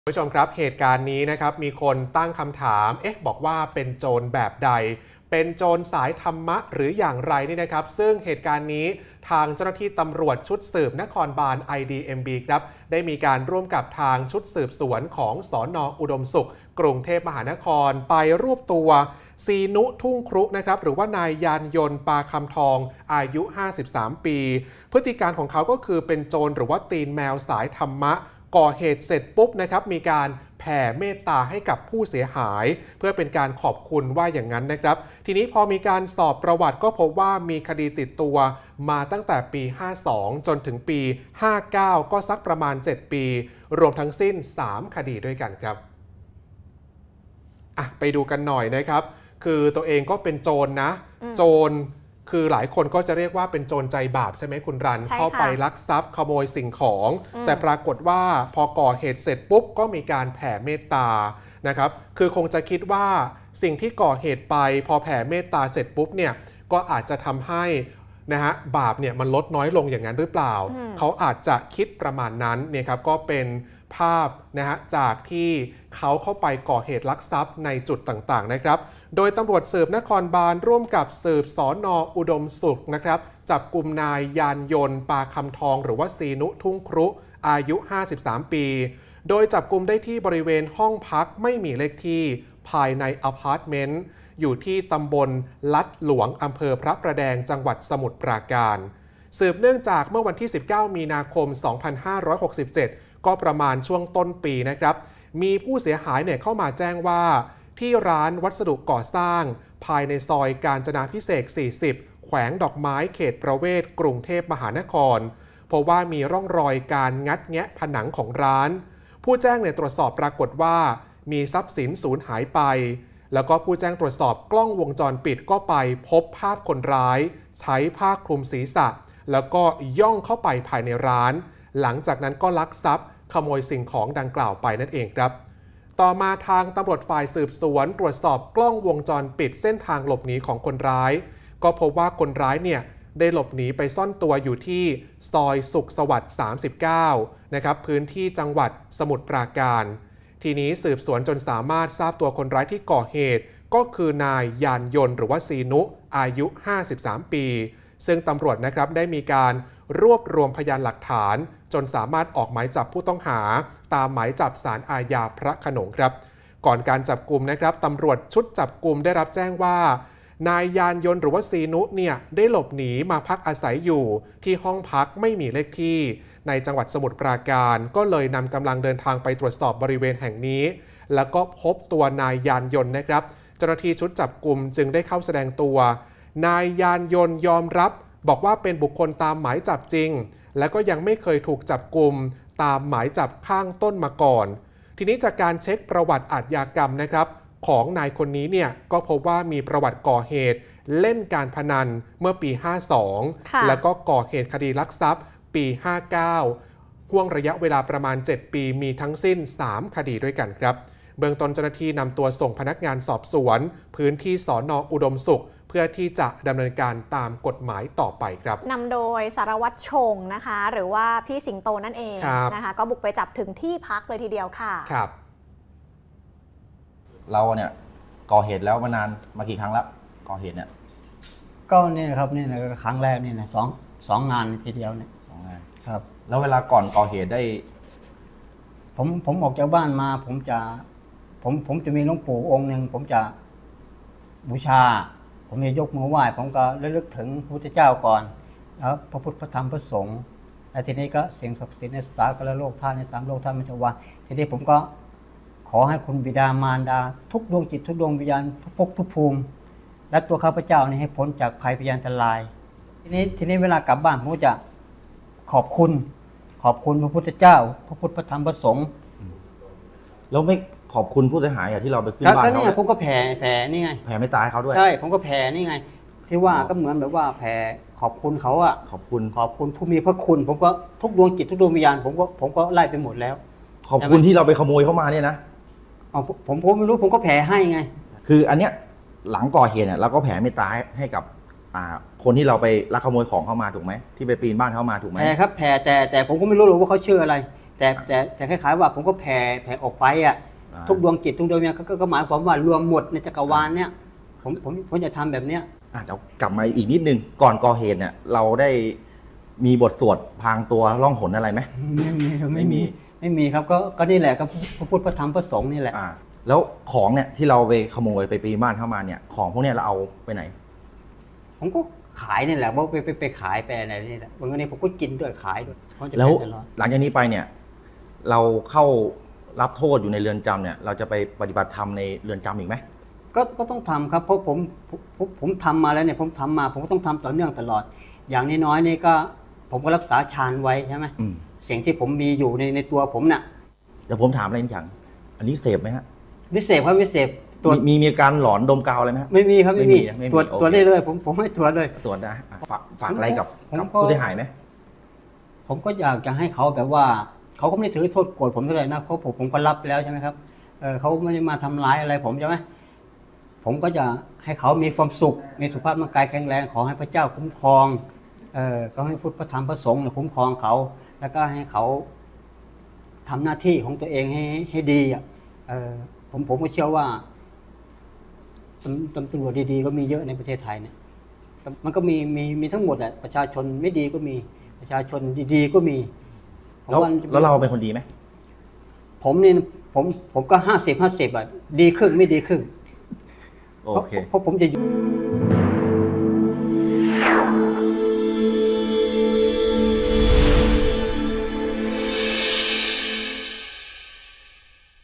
สืบนครบาล รวบ ซีนุ ทุ่งครุ ตีนแมวสายธรรมะก่อเหตุเสร็จแผ่เมตตาขอบคุณ | Hot Daily News 26 มิ.ย.67
สืบนครบาล ร่วม สืบ สน.อุดมสุข รวบ ซีนุ ทุ่งครุ ตีนแมวสายธรรมะ ก่อเหตุเสร็จแผ่เมตตาให้ผู้เสียหายเพื่อขอบคุณ สอบประวัติพบมีคดีติดตัวตั้งแต่ปี 52-59 รวม 3 คดี